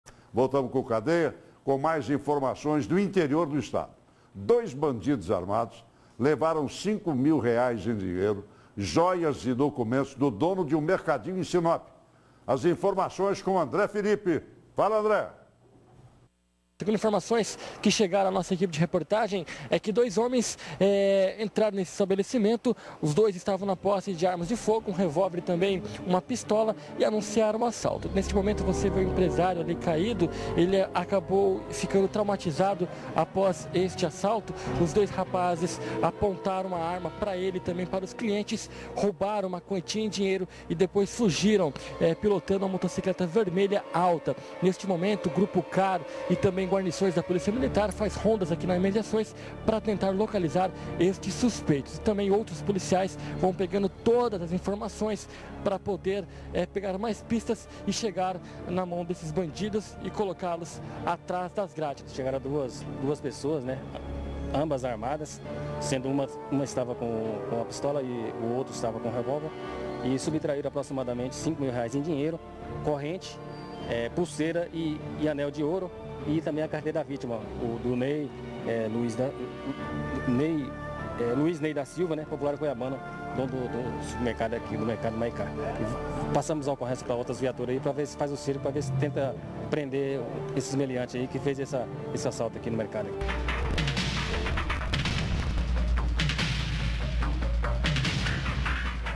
Bandidos assaltam comércio e ameaçam proprietário
Assaltantes fugiram levando dinheiro, jóias e documentos da vítima.